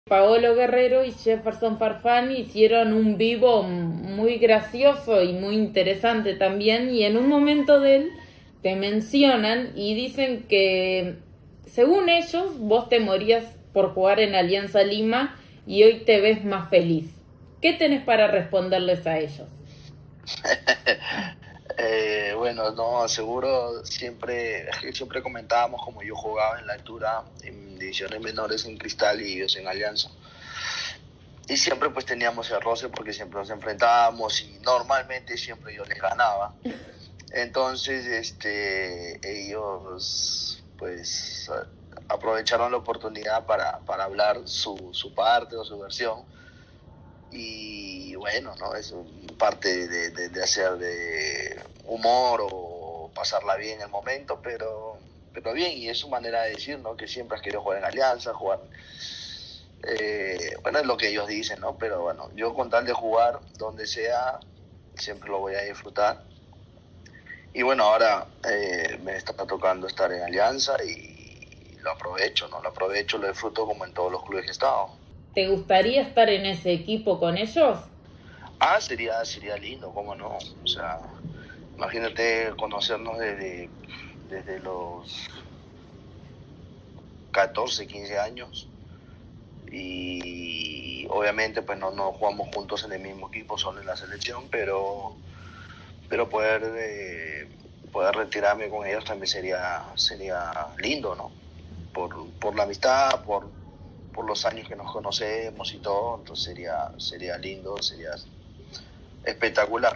Alberto Rodríguez le responde a Paolo Guerrero y Jefferson Farfán
El zaguero de Alianza Lima respondió lo que días atrás dijeron el 'Depredador' y la 'Foquita'.
Agregó que sería lindo retirarse con ellos en la institución íntima.